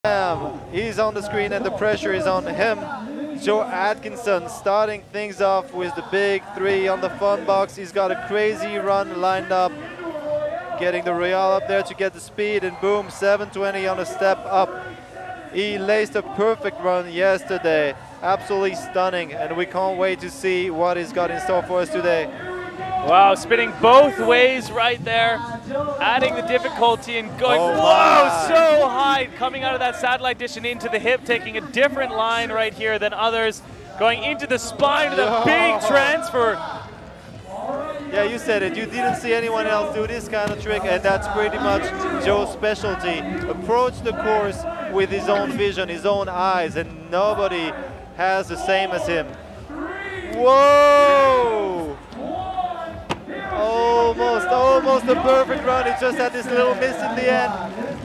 Joe Atkinson | 1st place - WS Roller Freestyle World Cup Final | FISE Hiroshima 2019
There's no upsetting the established order as the reigning champ tops the podium in the World Skate Roller Freestyle Final at FISEHiroshima.

1. Joe Atkinson 🇬🇧
2. Yasutoko Takeshi 🇯🇵
3. CJ Wellsmore 🇦🇺